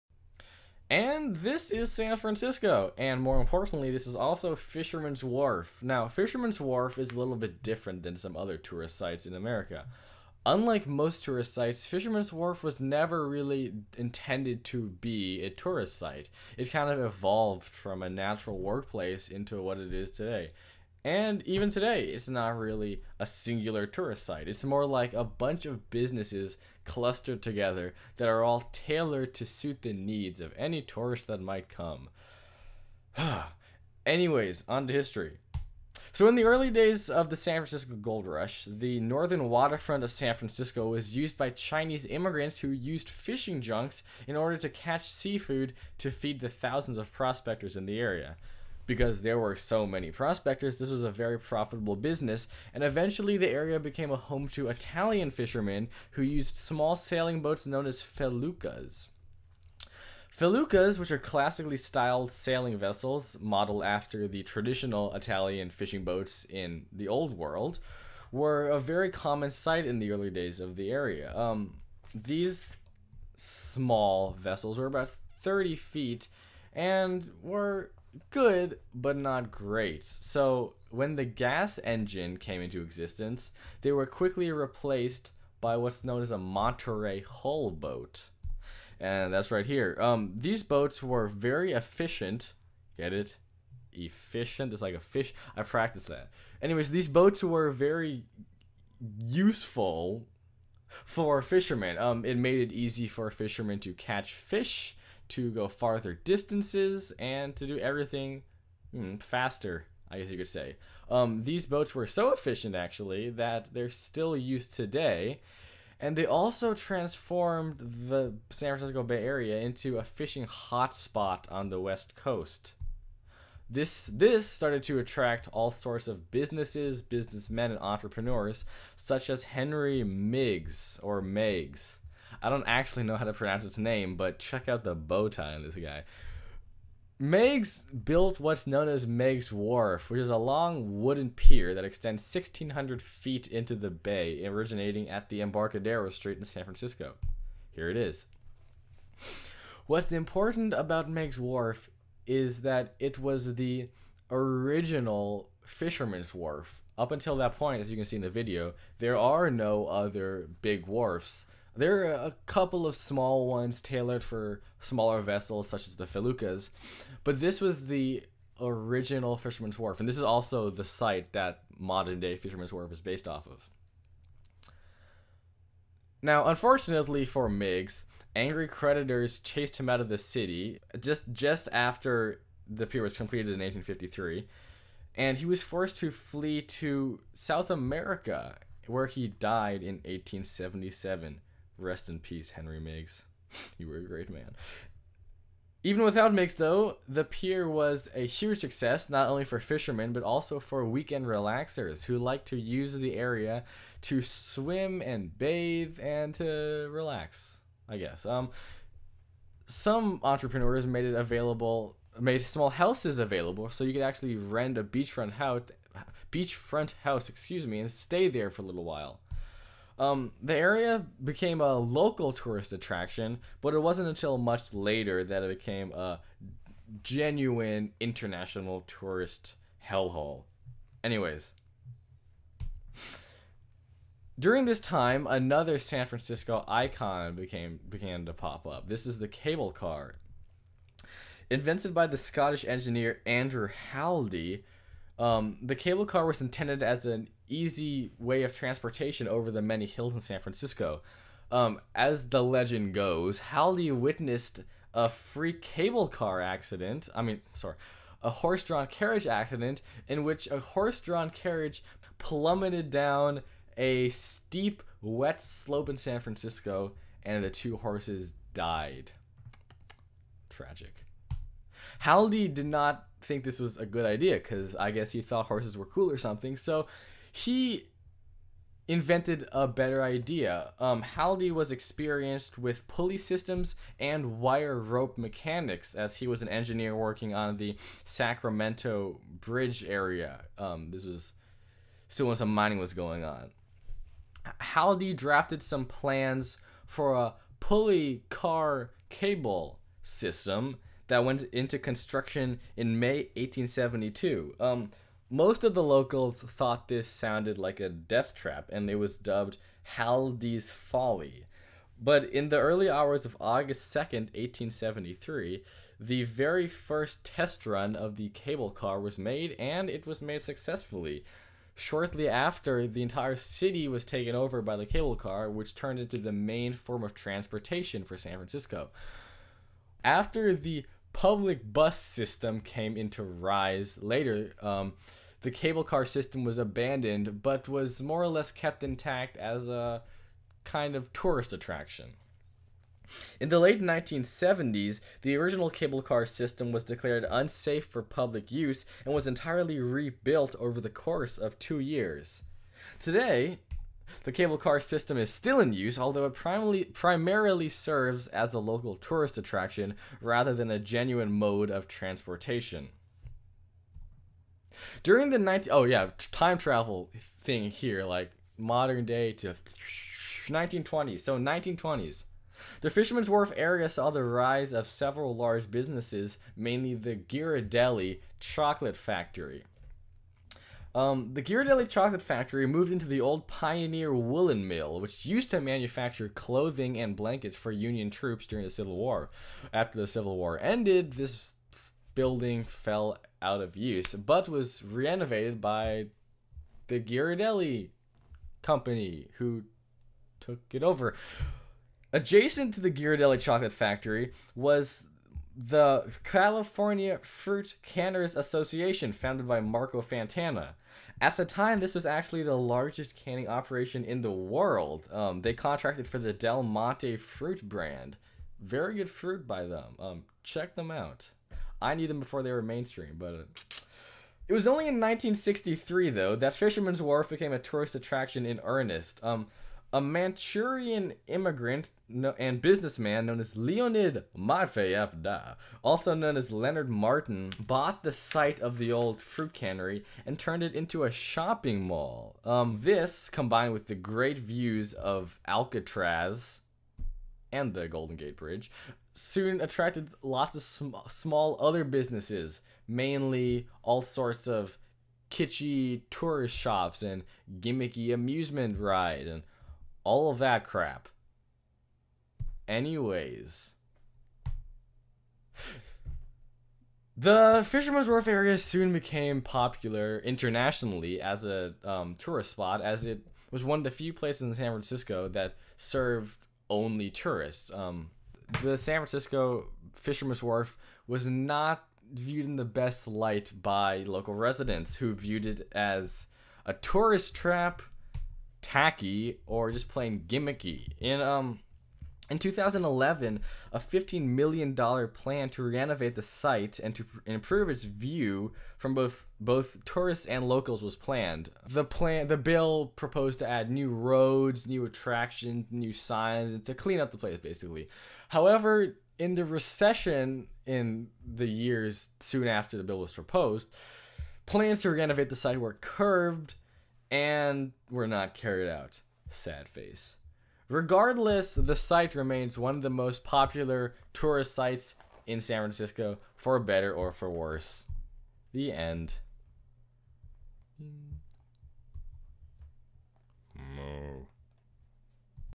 A Brief History of Fisherman's Wharf, SF
A brief history of San Francisco's Fisherman's Wharf (and of the cable car system nearby).
Image sources in video